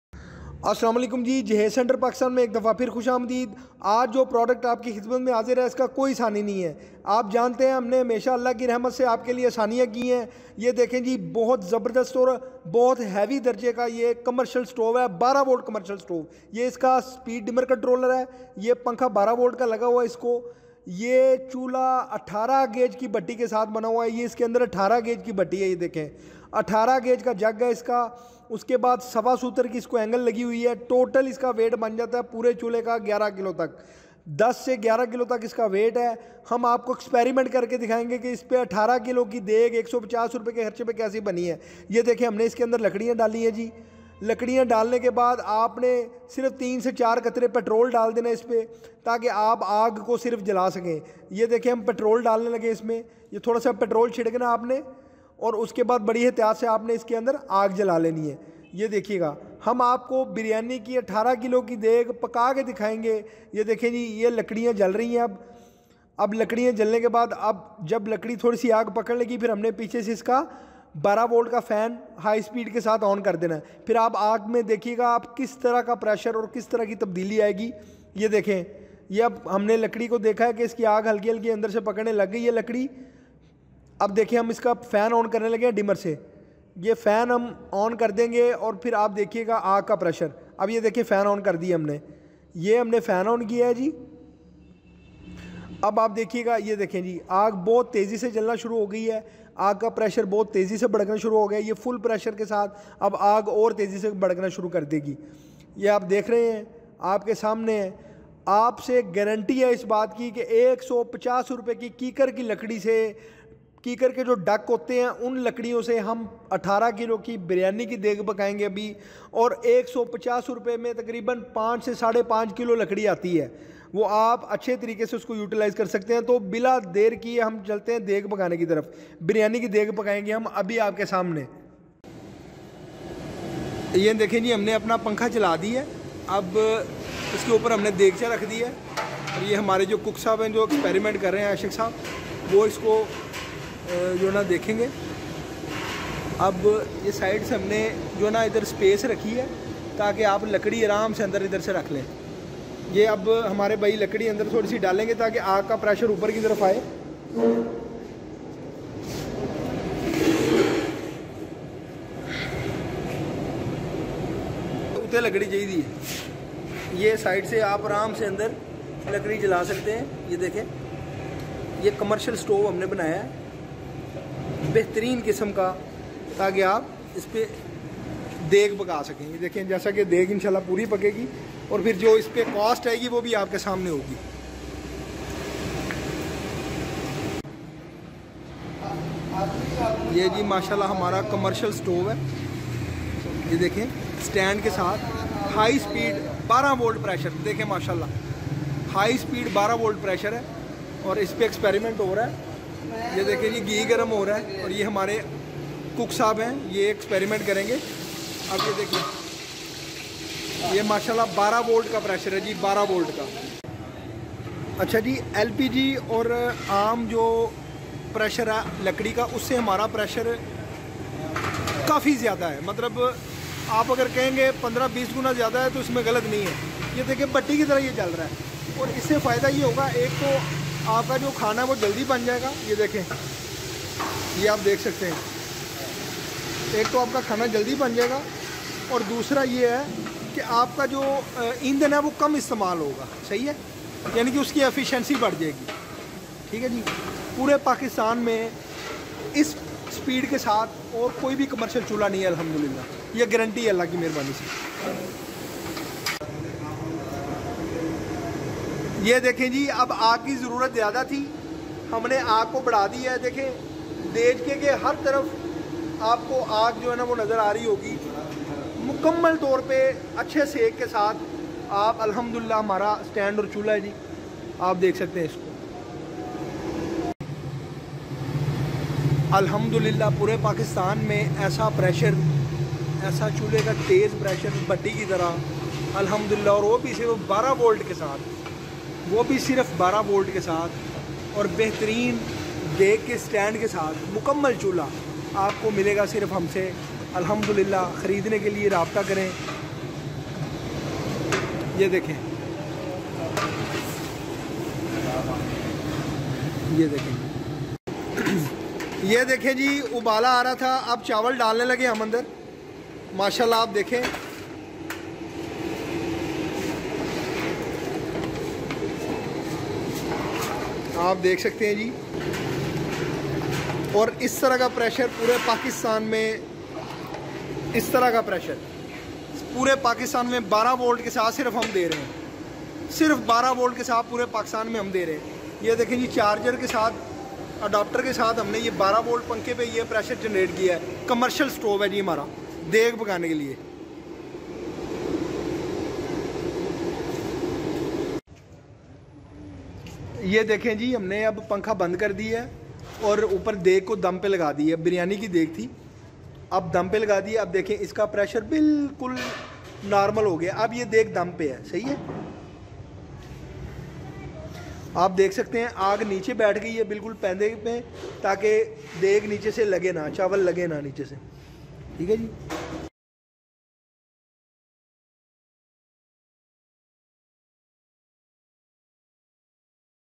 Suchal Deggi Commercial Stove | Booster Commercial Stove | Best Commercial Stove in Pakistan
This video is about the efficiency and usage of Suchal Deggi Commercial Stove which is also called Booster Commercial Stove is an invention of JAHEZ CENTER PAKISTAN, We guarantee and claim that this is the best commercial stove in Pakistan. We guarantee that only in 150 Rupees a commercial deg can be cooked in only 24 minutes. Viewers can see the process of using the stove.